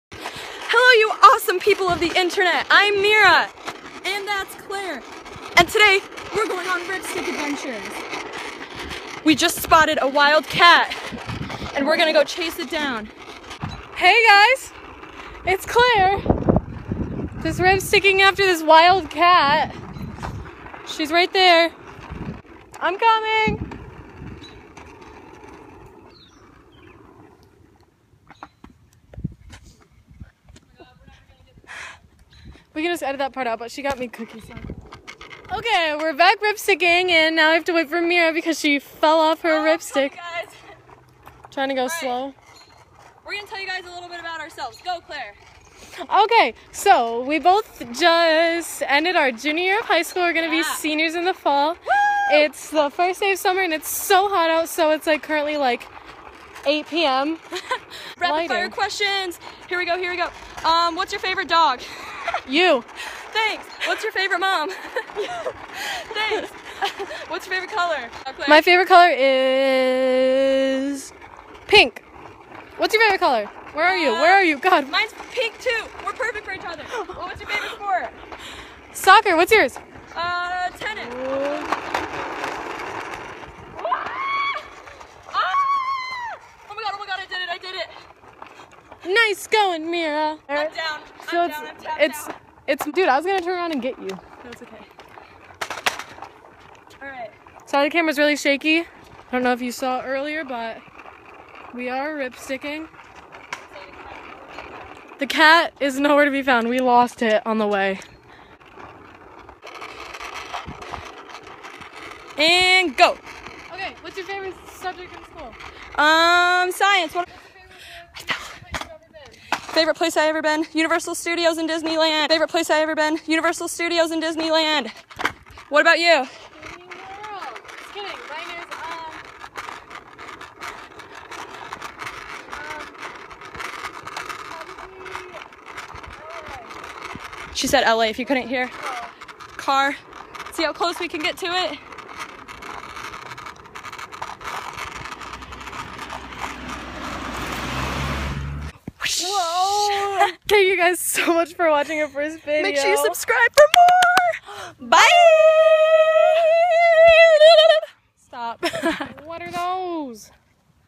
CHASING WILD CATS!! | first vlog
rip-sticking, questions, cars, and..cookies??

-mira+claire